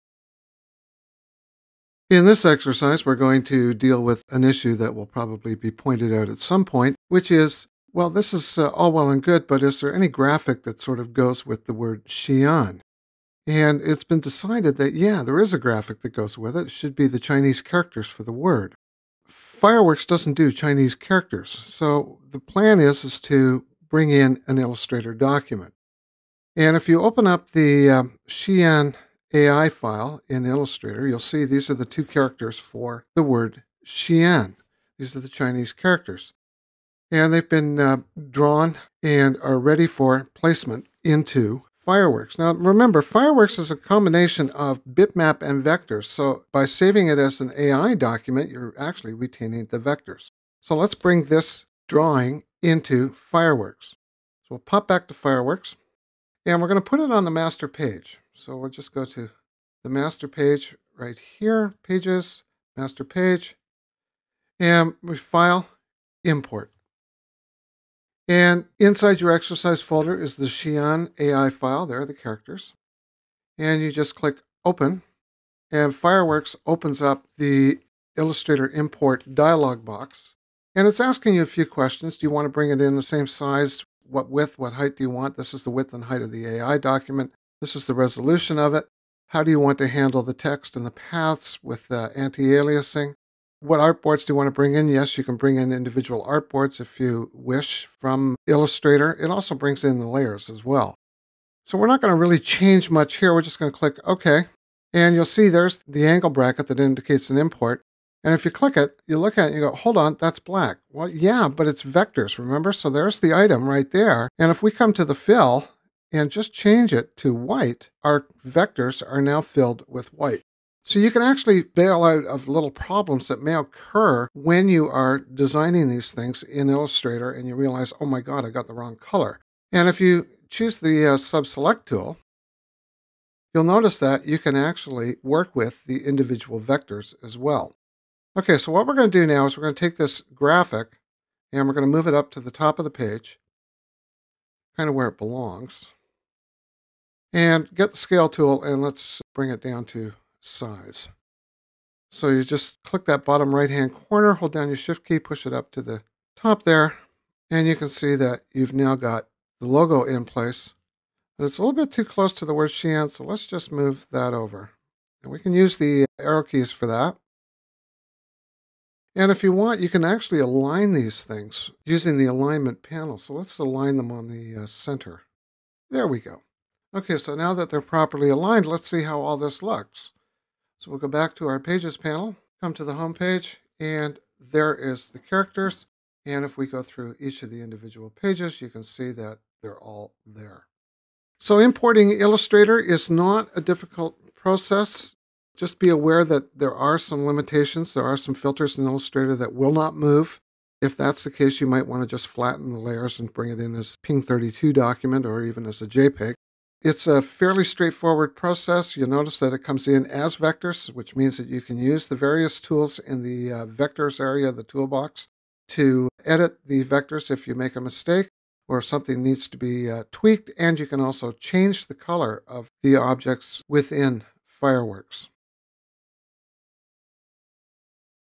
Fireworks CS5: Importing and editing Illustrator Documents
Fireworks uses vector and bitmap images. It makes sense, therefore, to assume that Illustrator and Fireworks have a working relationship. In this lesson you will learn how Illustrator artwork can be easily imported into Fireworks.